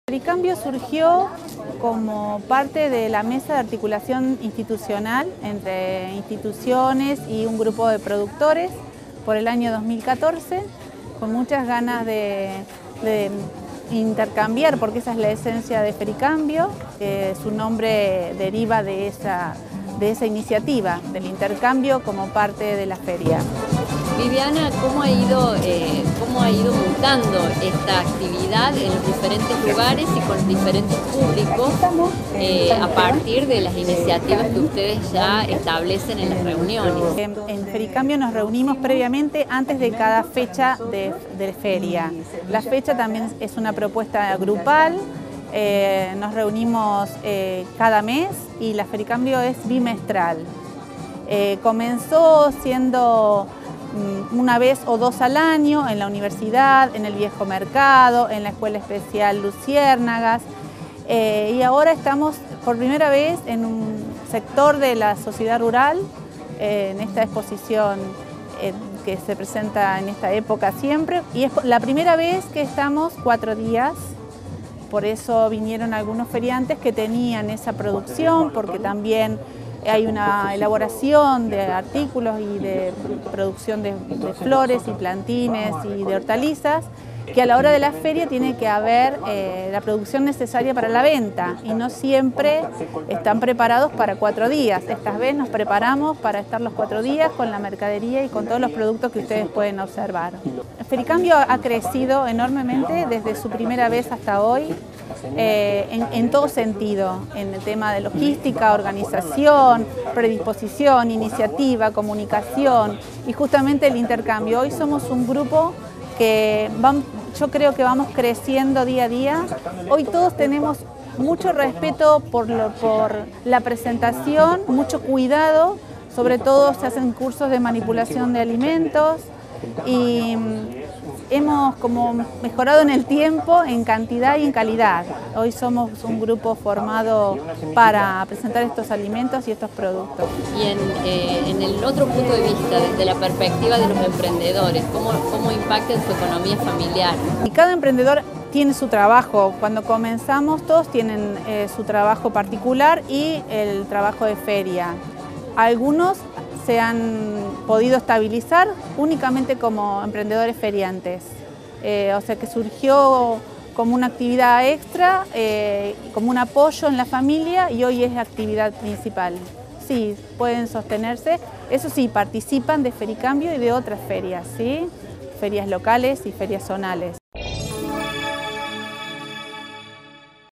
POR PRIMERA VEZ EL FERICAMBIO SE REALIZÓ EN LA EXPOSICIÓN RURAL DE RÍO CUARTO
BIBIANA SALAS - REPRESENTATE DE INTA
Facebook.com/uniriotv
Twitter.com/uniriotv